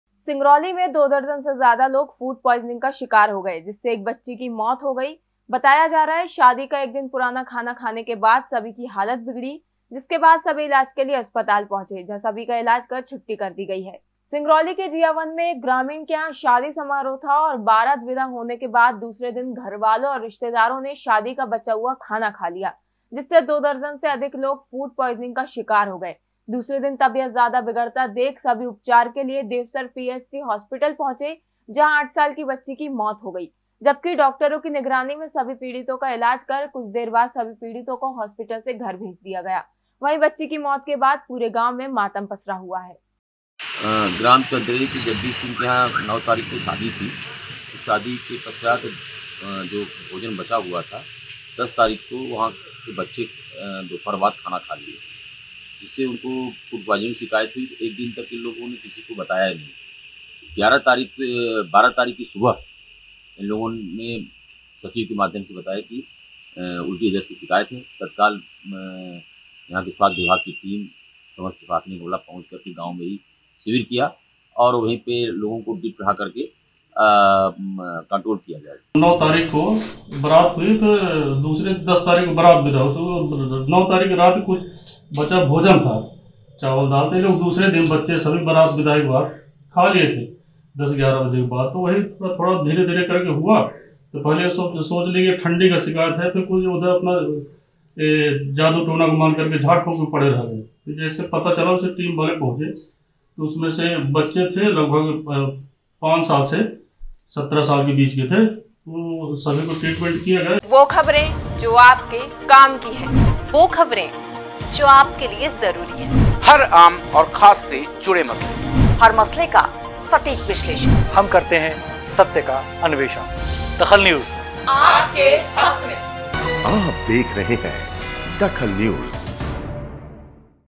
DAKHAL NEWS दो दर्जन लोग फ़ूड पॉइजनिंग का शिकार
दो दर्जन लोग फ़ूड पॉइजनिंग का शिकार
शादी का खाना खाकर लोग हुए बीमार